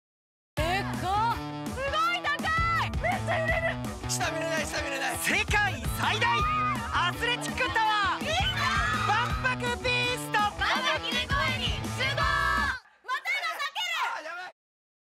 ボンボンTV・よっちの股が裂ける!?メンバーが全力でアスレチックを体験 「万博BEAST」CM「万博BEASTオープン」
ドイツ製巨大アスレチック施設「万博BEAST」が万博記念公園に15日オープンする。オープンに先駆けて、人気YouTubeチャンネル「ボンボンTV」にて、「万博BEAST」を体験した様子を映し出したタイアップ動画が11日公開された。